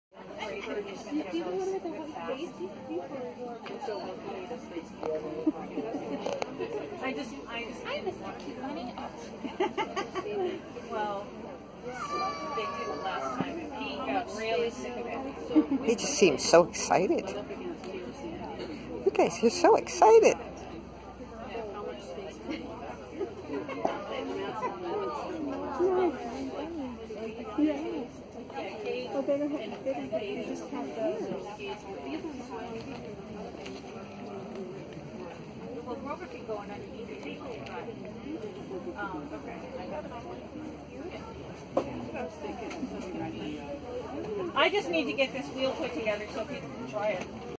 Angora rabbits at the Vermont Sheep and Wool Festival.
These guys looked so excited to be joining the fun at the festival.